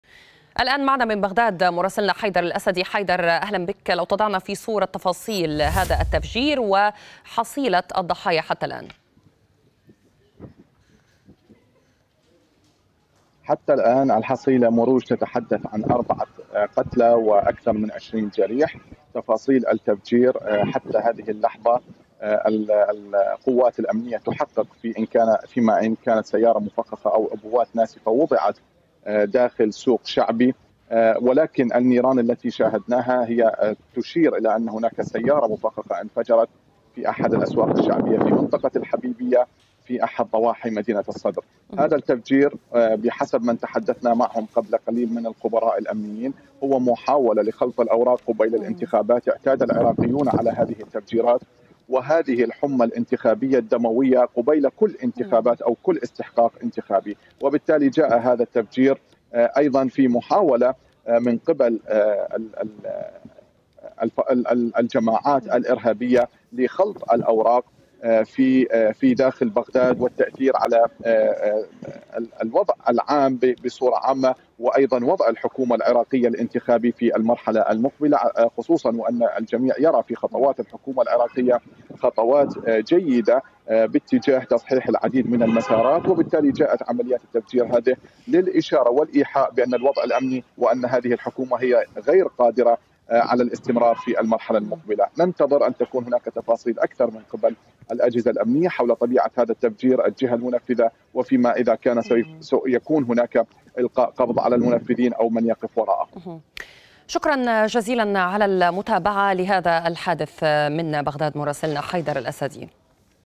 قتلى وجرحى في انفجار شرقي بغداد
مقتل أربعة أشخاص وإصابة عشرين في انفجار سيارة ملغومة بمدينة الصدر شرقي بغداد